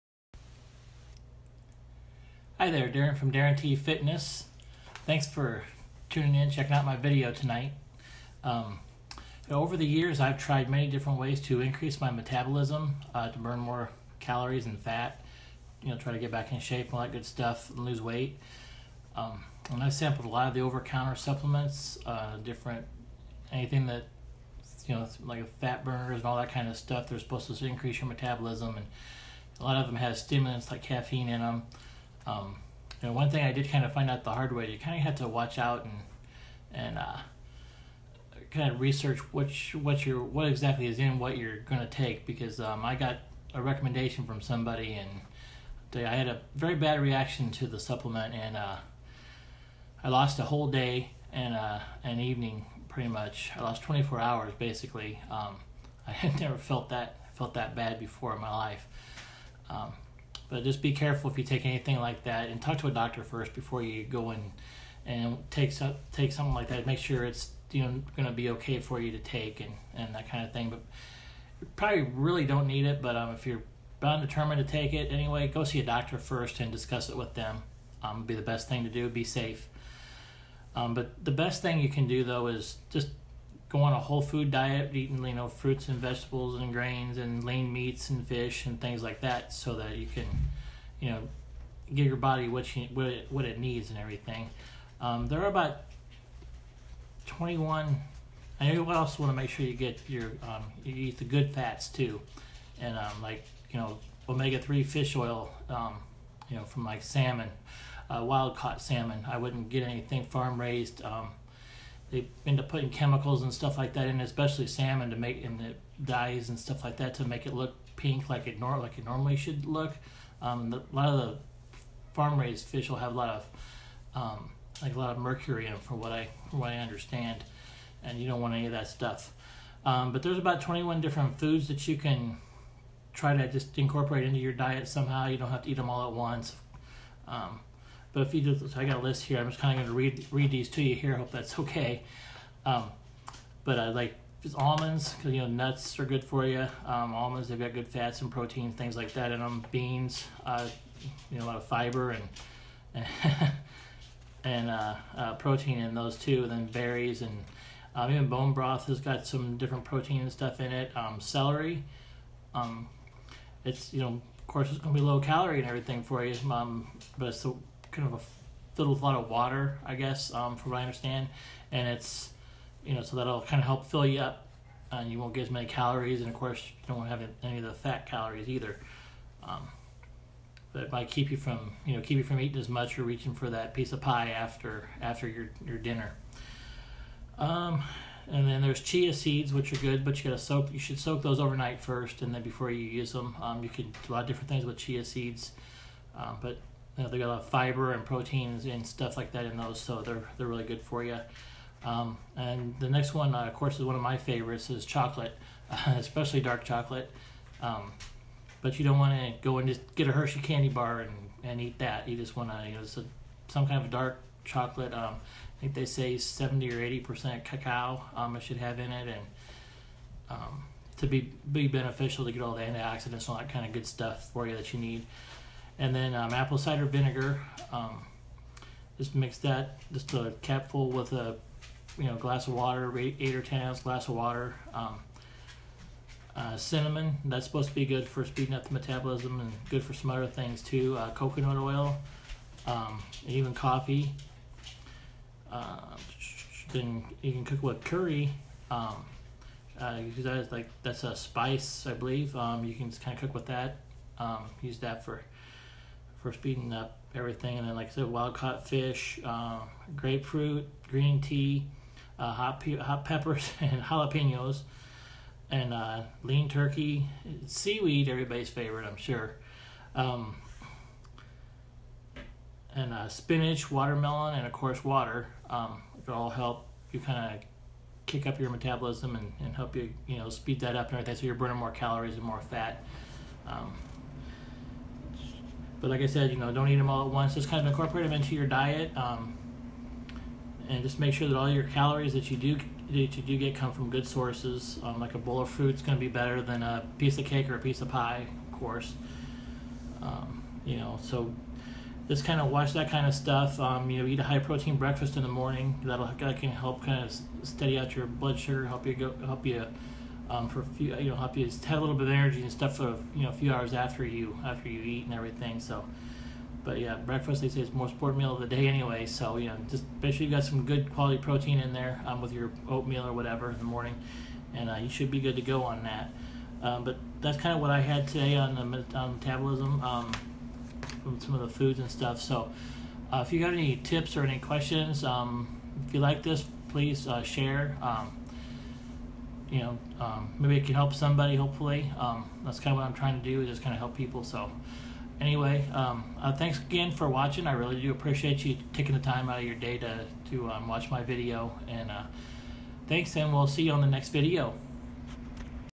How Do You Boost Your Metabolism
Over the years I have tried many ways to increase my metabolism to burn more calories and fat. Especially at Rest. I have sampled over the counter supplements that are supposed to increase the calorie burn. These usually have stimulants like caffeine in them. Check with your doctor first if you want to take these. Some are dangerous. I had a bad experience with one and lost a whole day. I have never felt so sick as I did that day. So be warned!

There are natural ways that are much safer to speed up your metabolism. There are no magic pills that are going to help you lose weight. It takes time, work and making healthy eating choices. There are supplements that will help you if you take them along with eating healthy and exercising. When you cut calories you also cut the nutrients out that the body needs to function properly.

You need to follow a whole food diet. Eat lots of fish, lean meat, fruits, veggies and fats. Yes, I said eat fat. There is good fat that the body needs. Like Omega-3 fish oil. Make sure that the fish you eat is wild caught and not farm raised. Farm raised usually has chemicals added to make the fish look normal.

Foods that help increase your metabolism